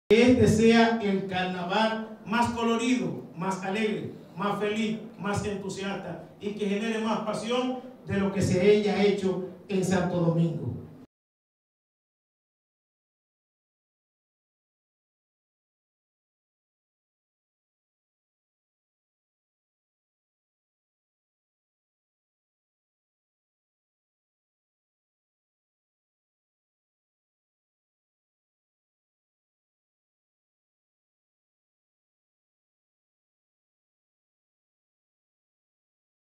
ASDN anuncia celebración del Carnaval 2022 en avenida Hermanas Mirabal
La Alcaldía de Santo Domingo Norte (ASDN) anunció que celebrará un colorido y original desfile de Carnaval 2022 el domingo 27 de marzo, iniciando a las 2:00 pm desde el Torito de Villa Mella hasta el parque Máximo Gómez, en la avenida Hermanas Mirabal.

En un comunicado de prensa, el cabildo informó que a ritmo carnavalesco coronó este viernes al profesor Ramón Rosario Coco (Monchi) como rey y a la cantautora de música folclórica, Eneroliza Núñez Ferran, como reina del tradicional Desfile del Carnaval de Santo Domingo Norte, en un breve acto encabezado por el alcalde Carlos Guzmán.

La Alcaldía destacó que está edición del Carnaval SDN 2022 es dedicada al ícono de la Cultura de este municipio, el docente Víctor Julio Sánchez Piñeyro.